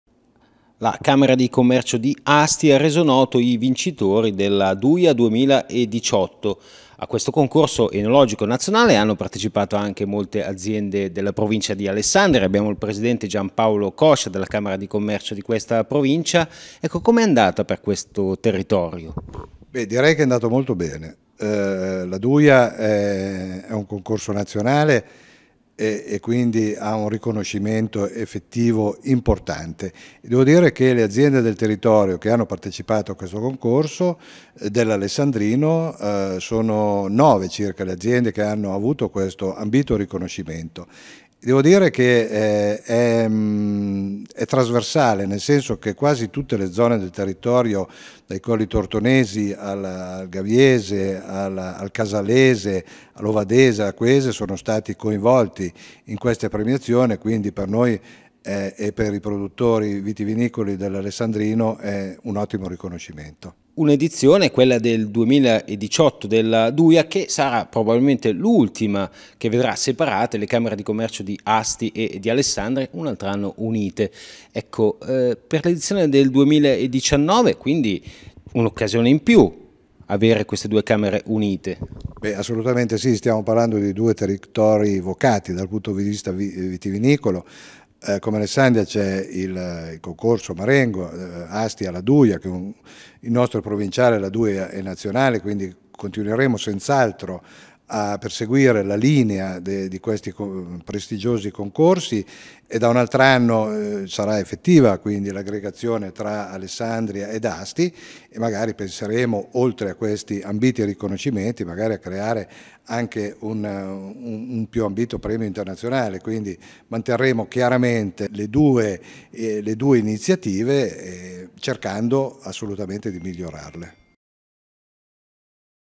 ALESSANDRIA: Nove aziende premiate con la Douja - 25.07.2018 GRP tv
Sono nove le aziende vitivinicole dell'alessandrino premiate dalla Camera di Commercio di Asti con la Douja 2018.